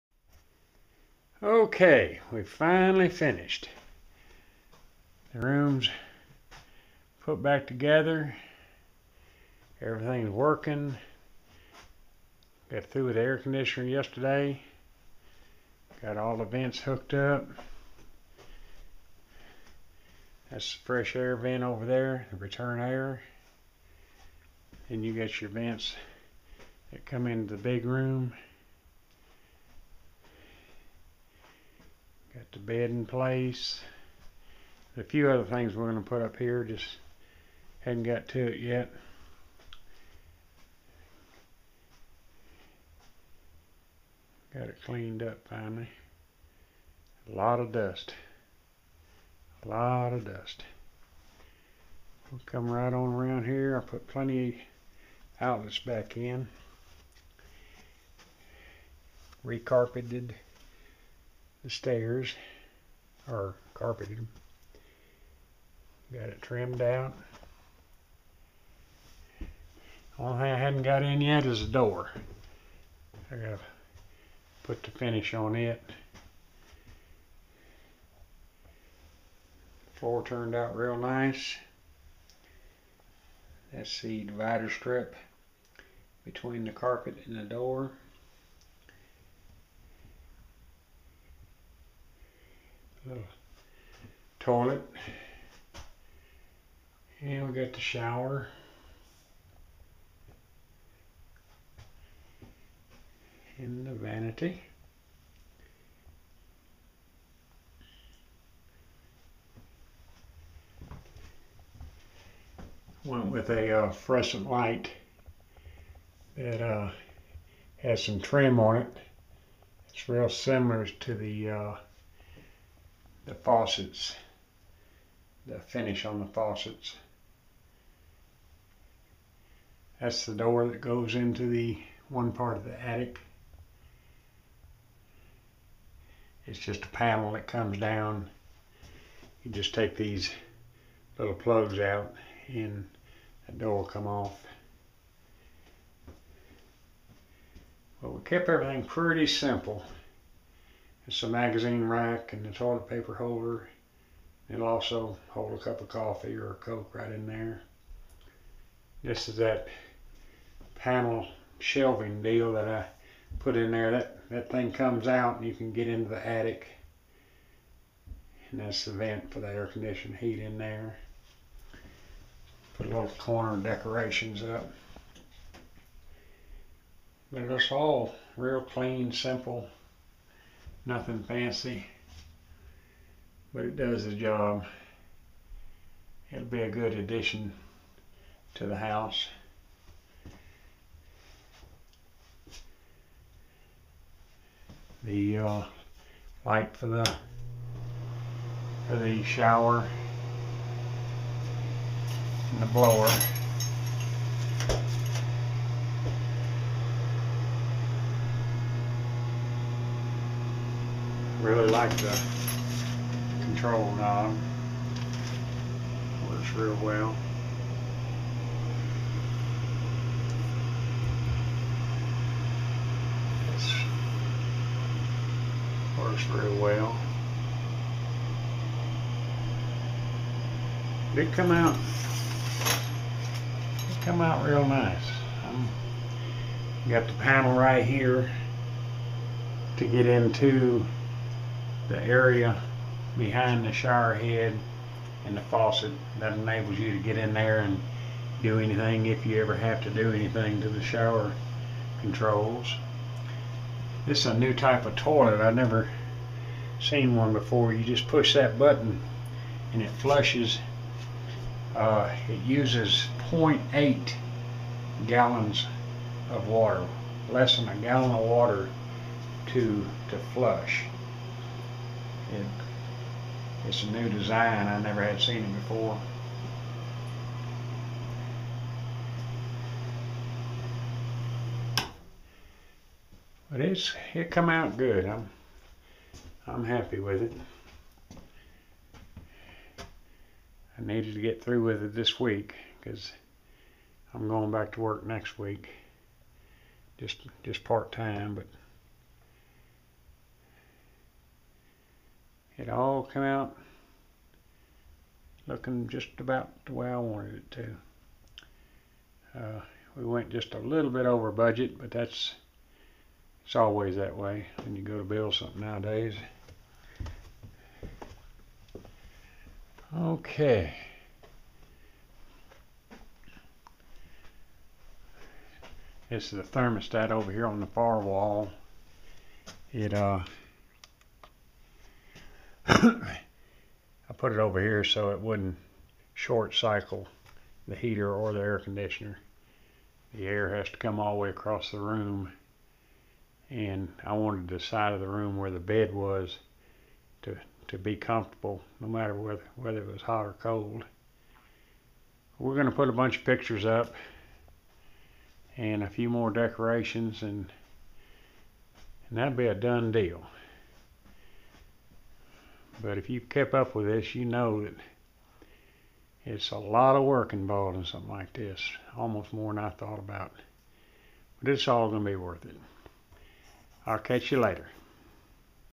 100_0002.MOV Check it out - poject is finished
Bathroom addition is finally finished - time to go fishing  :-)